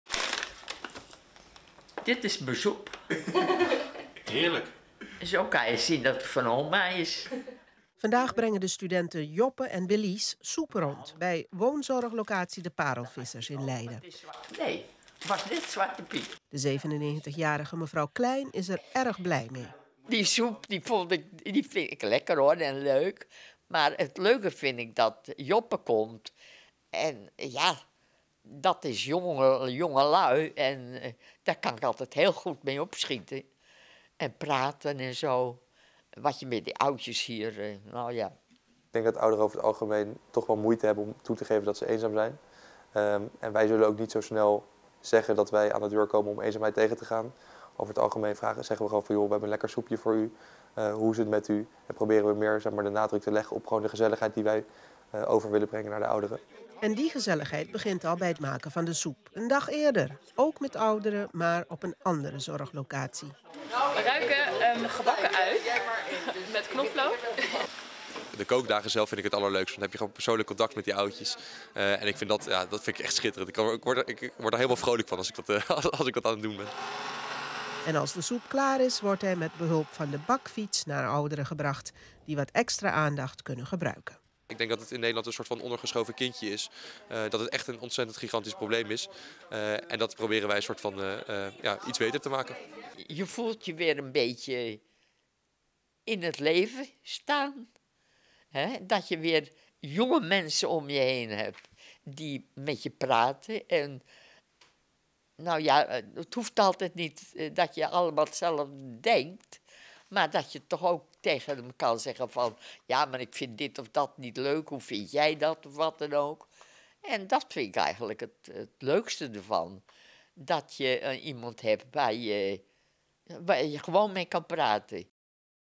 Studenten maken Oma's Soep (6 december 2021).
Studenten van Minerva maken soep voor ouderen bij Oma's Soep Leiden.